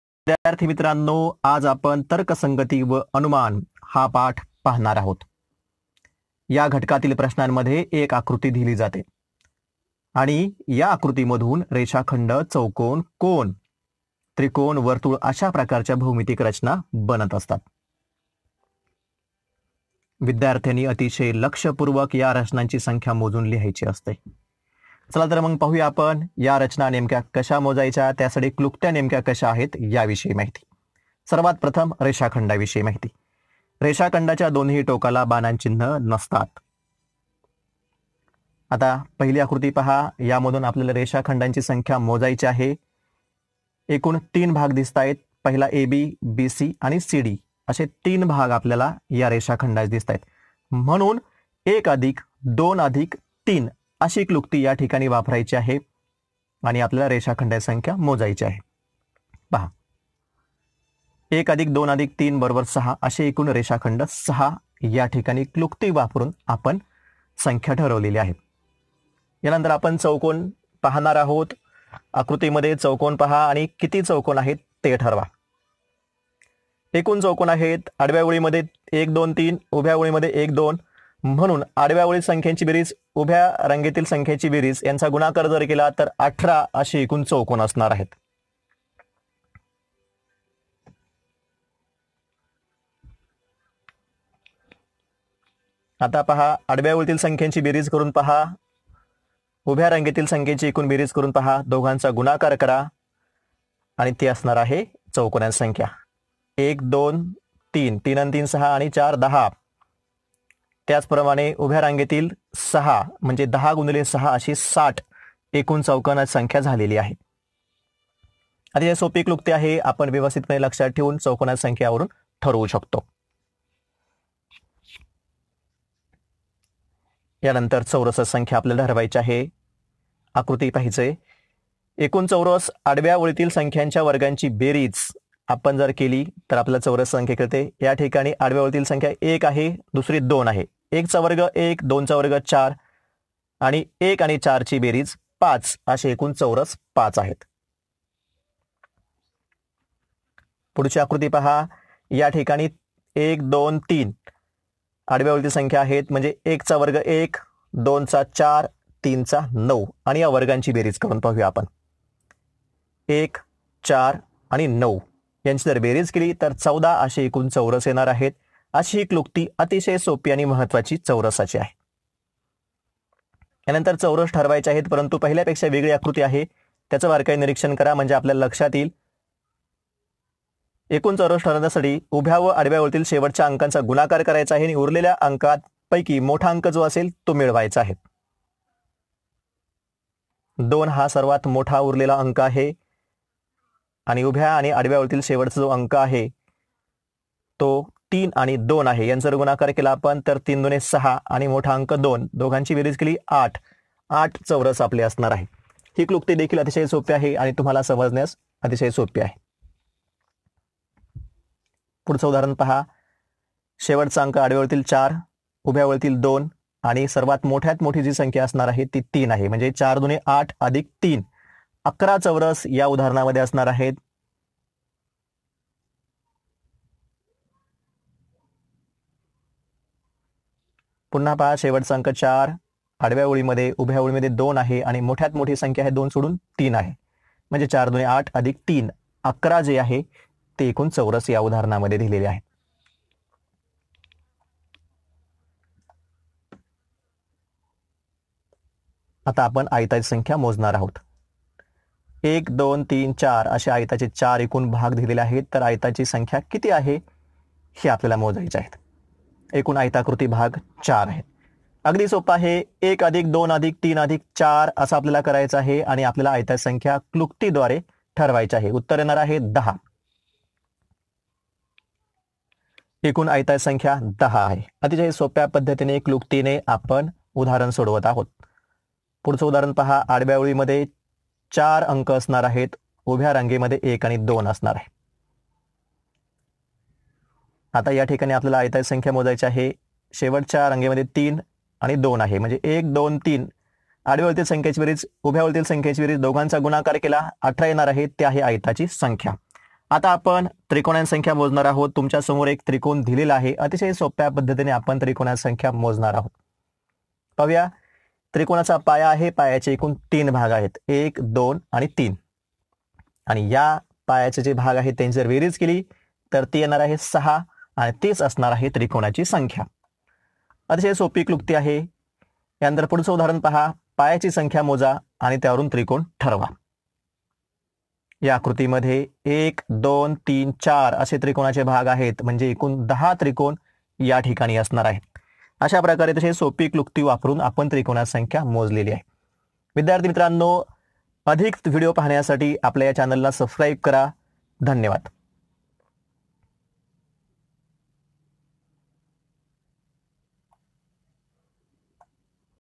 NMMS EXAM विषय: बुद्धिमत्ता (आकृत्यांची संख्या मोजणे)
विषय: बुद्धिमत्ता
घटक: तर्कसंगती व अनुमान (आकृत्यांची संख्या मोजणे)